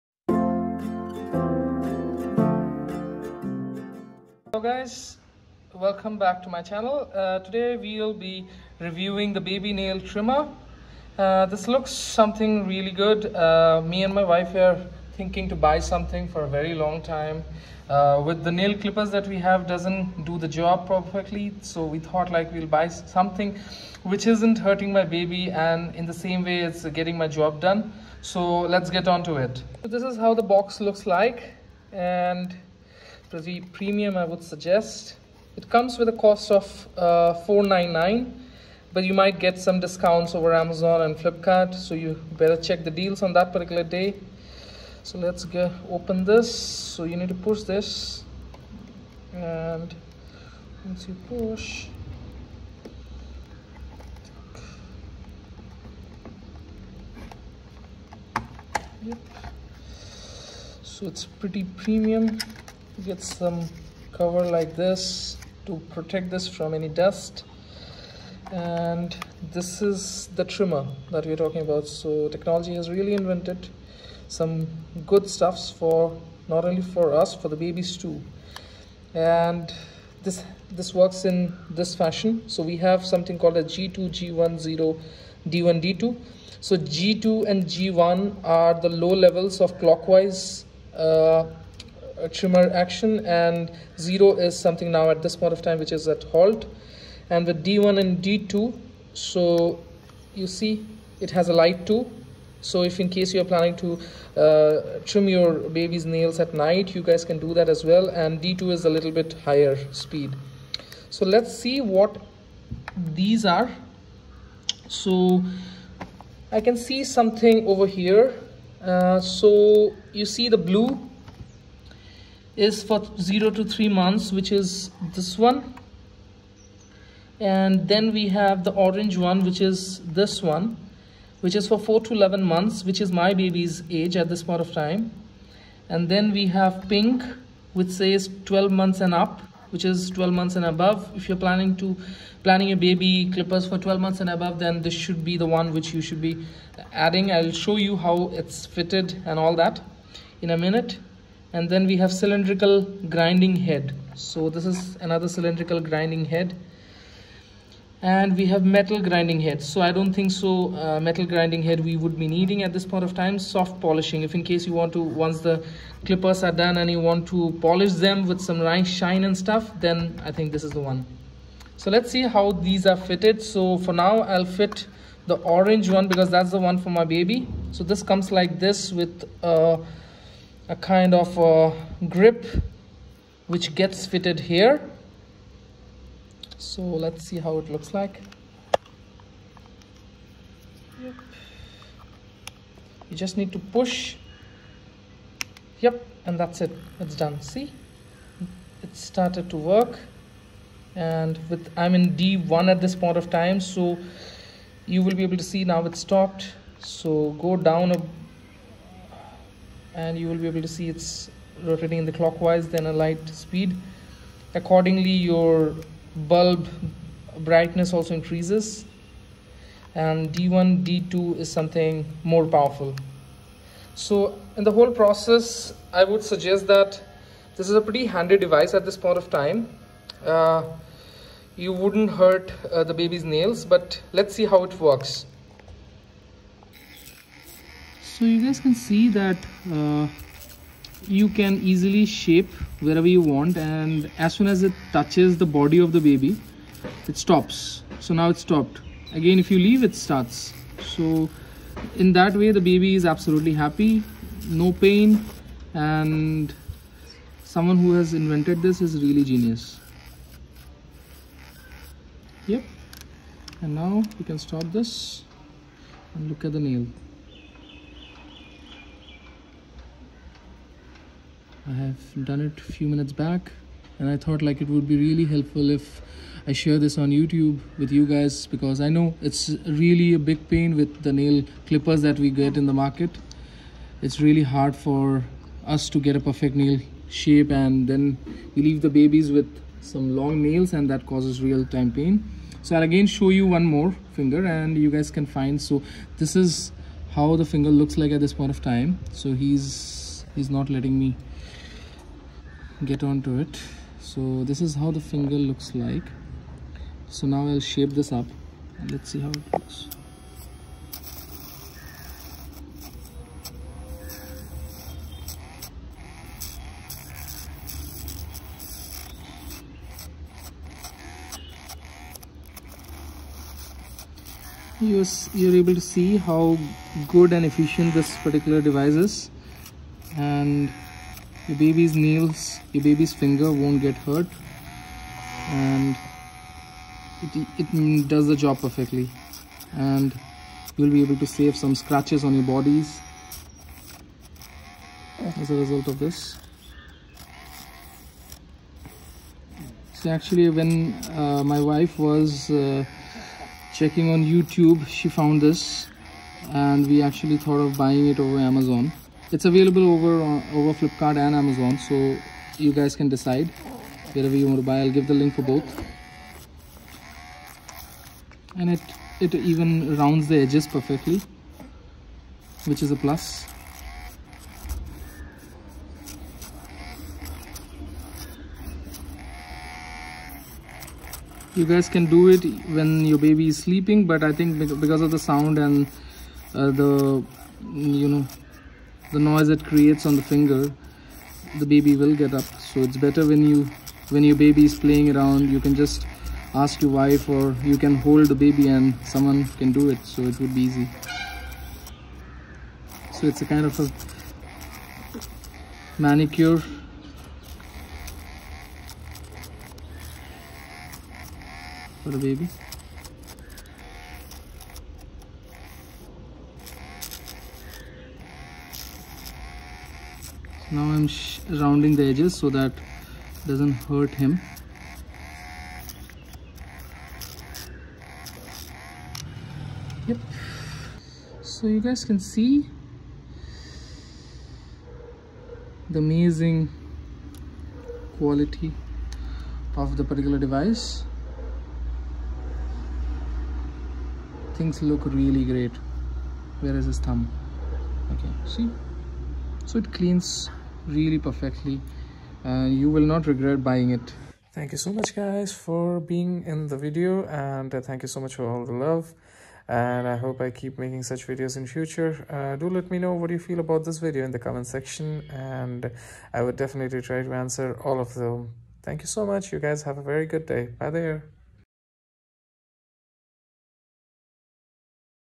Baby Nail Trimmer, Battery Operated. Cheapest Nail Clipper Ever!!
Showell Baby Nail Trimmer

Amazon link : KTPlus Baby Nail Trimmer File Electric Safe Nail Clippers with Light for Newborn or Toddler Toes and Fingernails

Battery: AA

Modes :
Blue : 0-3 Months
Orange : 4-11 Months
Pink : 12 Months +
Cylindrical : For shaping edges
Metal Grinding : For hard skin
Soft Polishing : Polishing & Shining nails